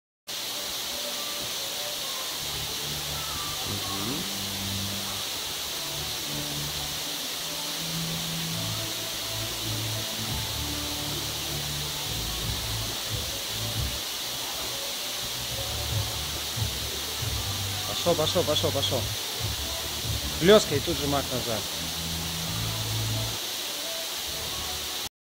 Спорт и выживание. Фрагмент тренировки
фрагмент тренировки, Ростислав и нунчаки)